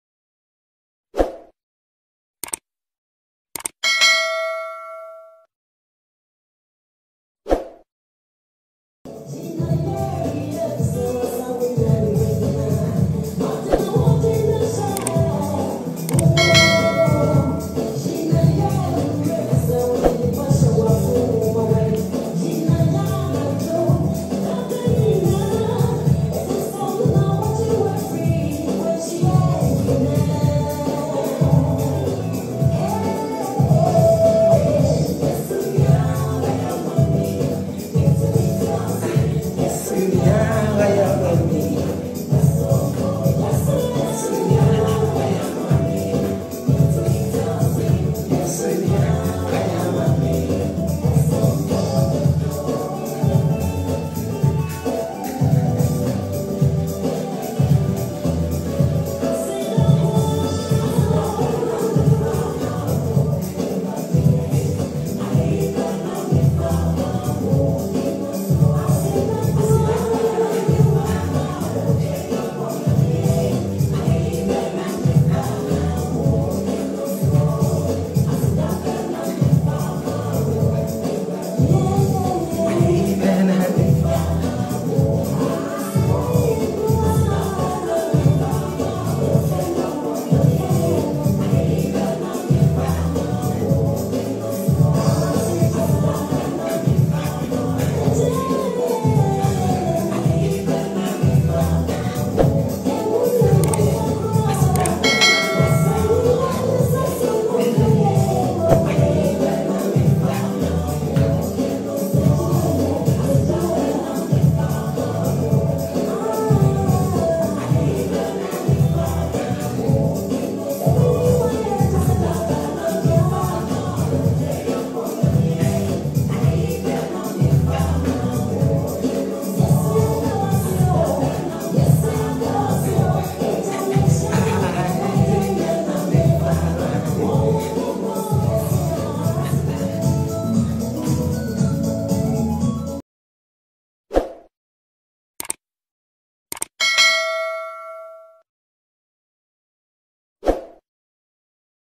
Eeii🔥😲Abruku Abruka replies to Kennedy Agyapong, Kwaku Annan & Manasseh after fake Prophet exposé
Abruku Abruka replies to Kennedy Agyapong, Kwaku Annan & Manasseh hilariously after expose

lets vibe !